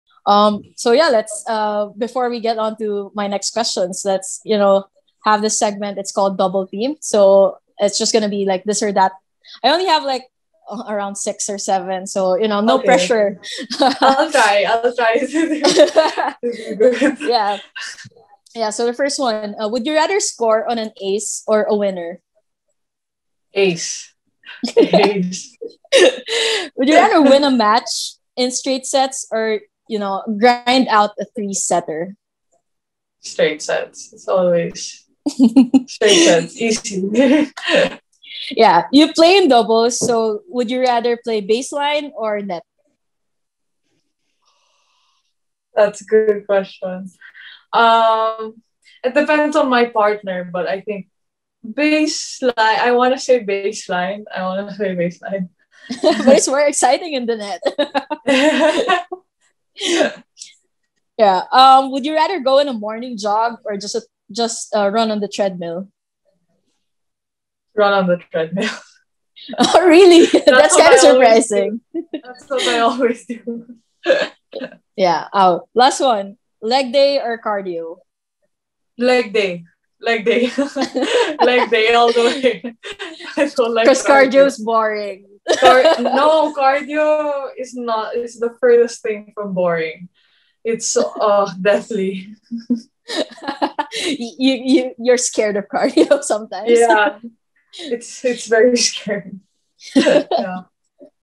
Double Team with Alex Eala
As she is now on her way to compete at the Wimbledon Championships, get to know tennis' teenage prodigy Alex Eala with these quick questions.
Video by Philstar.com